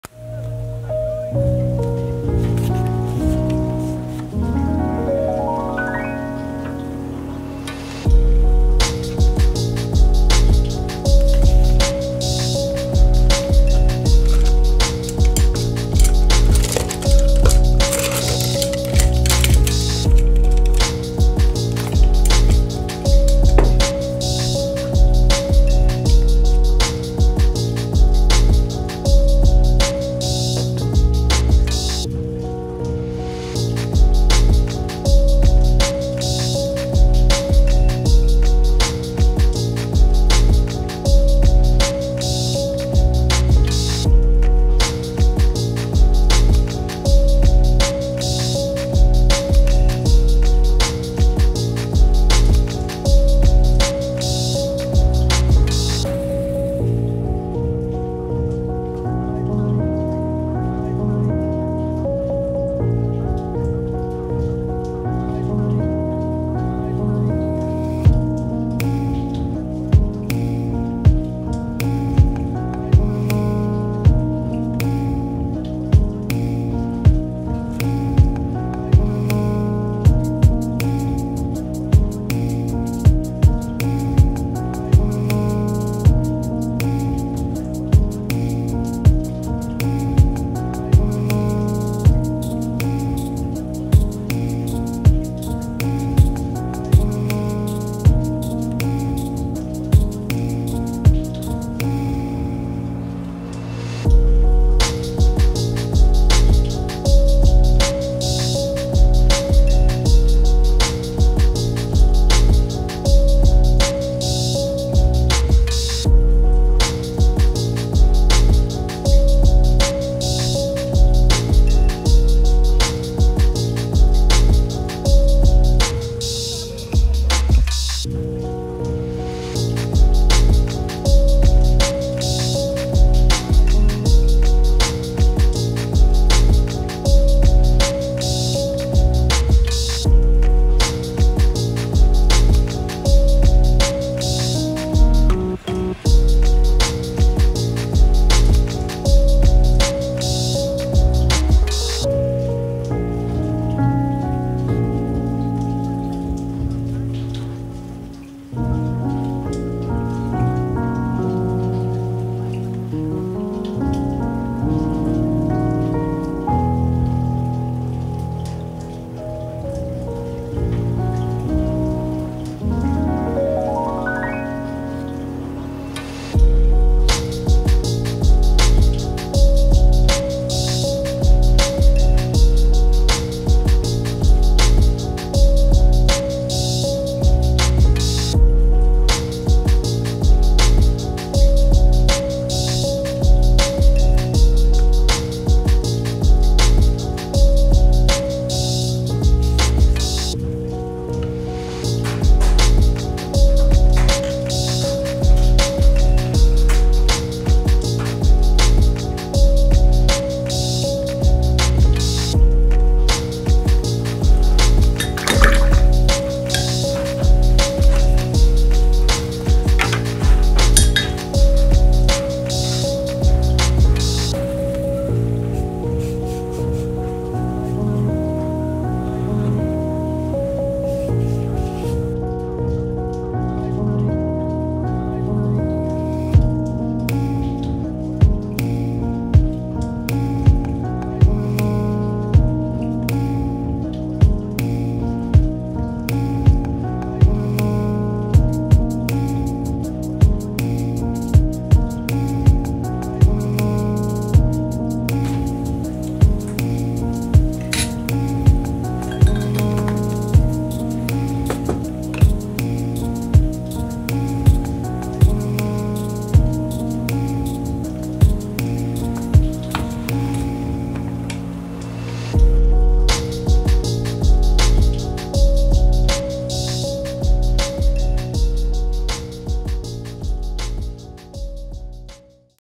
SKETCHING // Ballpoint Pen Portrait + Sumi Ink
Quick sketch done with ballpoint pen and black sumi ink